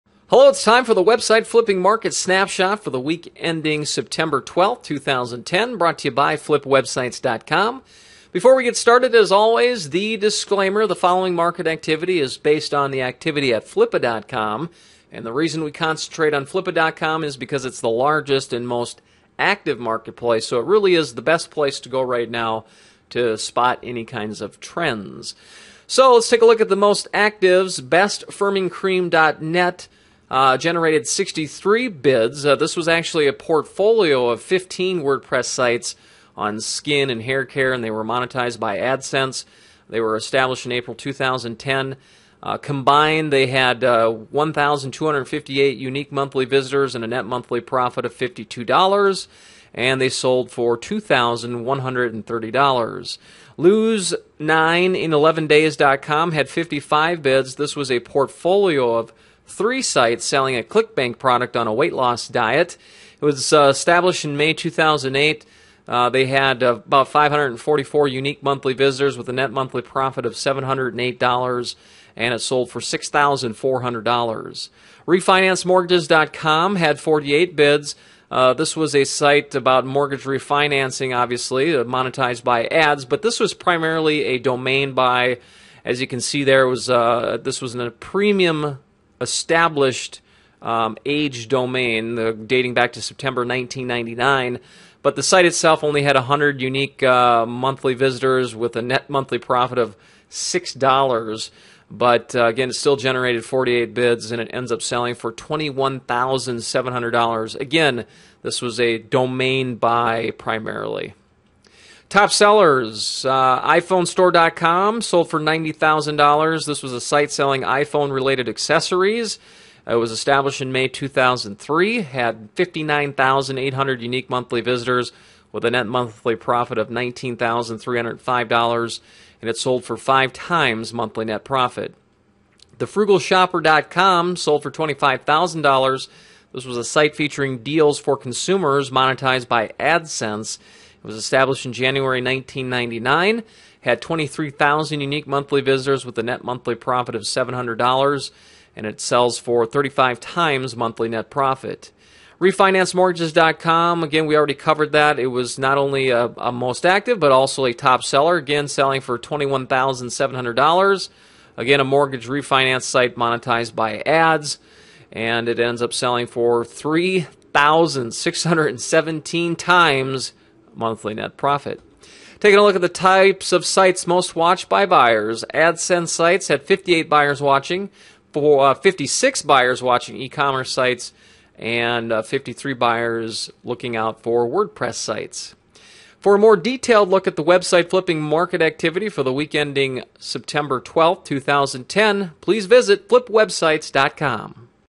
Website Flipping Stat: iPhoneStore.com Sells For $90,000
A portfolio of 15 AdSense sites was the most active website auction with 63 bids. iPhoneStore.com was an ecommerce store that was the top seller that sold for $90,000.